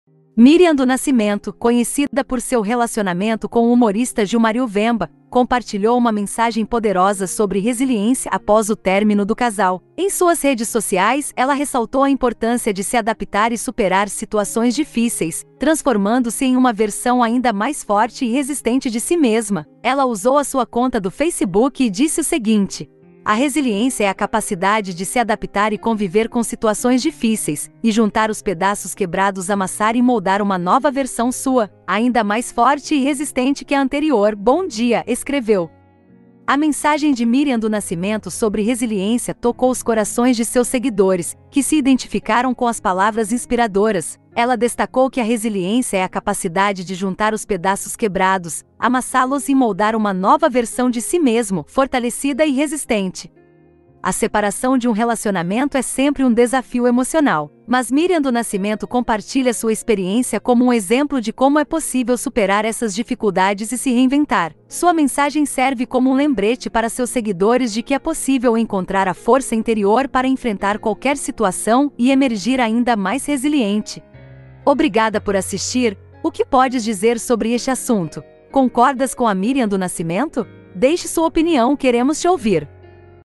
EX ESPOSA DO GILMÁRIO QUEBRA O SILÊNCIO E FALOU SOBRE O FIM DO CASAMENTO '' JUNTAR OS PEDAÇOS ''
Miriam do Nascimento, conhecida por seu relacionamento com o humorista Gilmario Vemba, compartilhou uma mensagem poderosa sobre resiliência após o término do casal, Em suas redes sociais, ela ressaltou a importância de se adaptar e superar situações difíceis, transformando-se em uma versão ainda mais forte e resistente de si mesma, ela usou a sua conta do facebook e disse o seguinte,

A resiliência è a capacidade de se adaptar e conviver com situações difíceis , e juntar os pedaços quebrados amassar e moldar uma nova versão sua , ainda mais forte e resistente que a anterior, bom dia, Escreveu

A mensagem de Miriam do Nascimento sobre resiliência tocou os corações de seus seguidores, que se identificaram com as palavras inspiradoras, Ela destacou que a resiliência é a capacidade de juntar os pedaços quebrados, amassá-los e moldar uma nova versão de si mesmo, fortalecida e resistente,

A separação de um relacionamento é sempre um desafio emocional, mas Miriam do Nascimento compartilha sua experiência como um exemplo de como é possível superar essas dificuldades e se reinventar, Sua mensagem serve como um lembrete para seus seguidores de que é possível encontrar a força interior para enfrentar qualquer situação e emergir ainda mais resiliente,